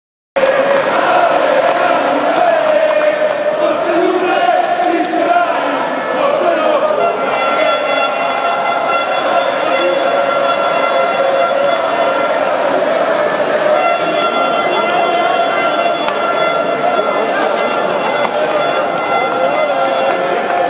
Ultras Juventus fuera del Bernabéu HD. Real Madrid - Juventus (13/14)
23 de octubre de 2013. Estadio Santiago Bernabéu. Real Madrid 2-1 Juventus